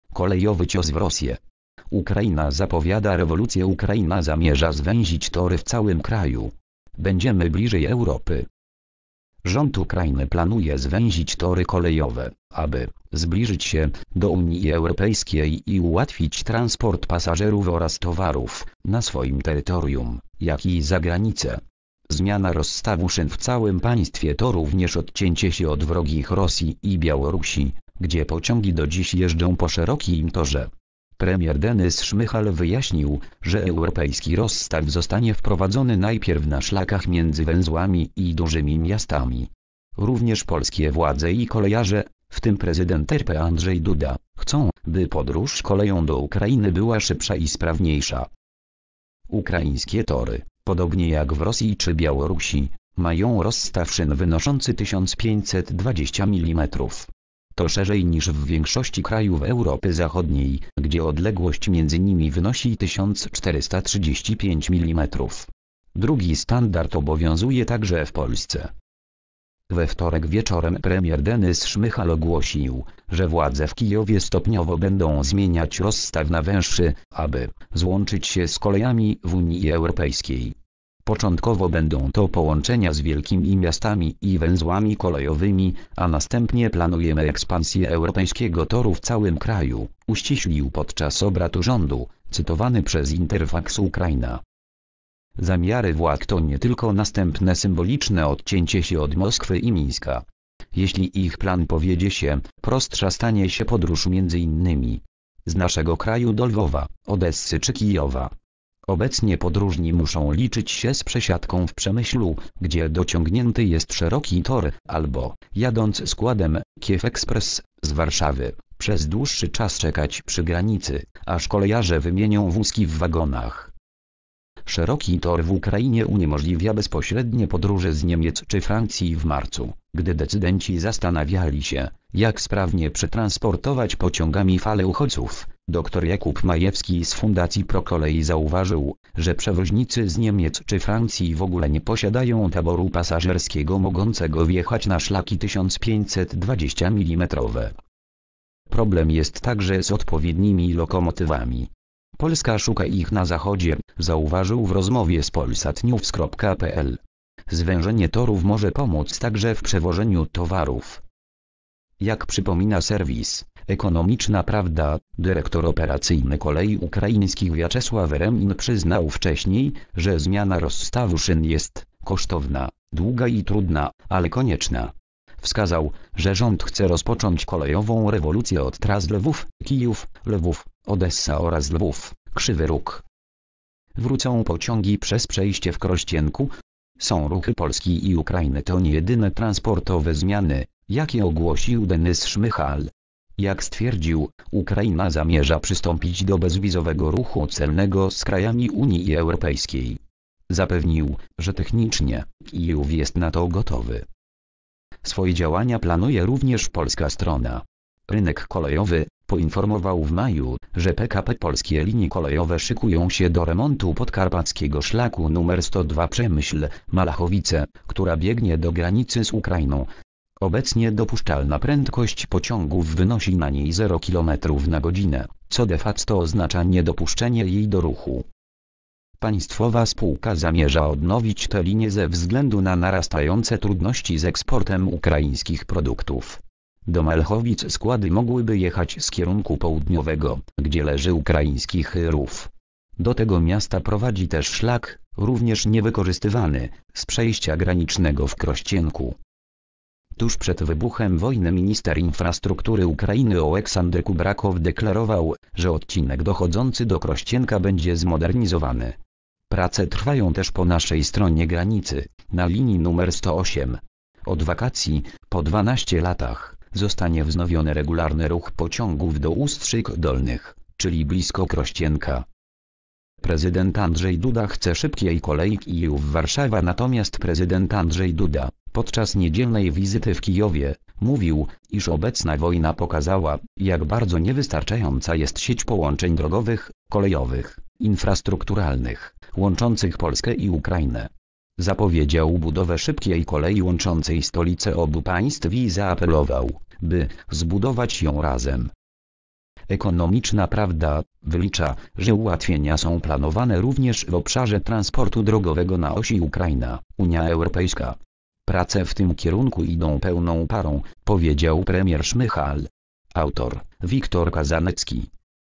Kolejowy cios w Rosję. Ukraina zapowiada rewolucję.
Ukraina zamierza zwęzić tory w całym kraju. "Będziemy bliżej Europy"

  (fot. Beata Zawrzel/NurPhoto/Getty Images)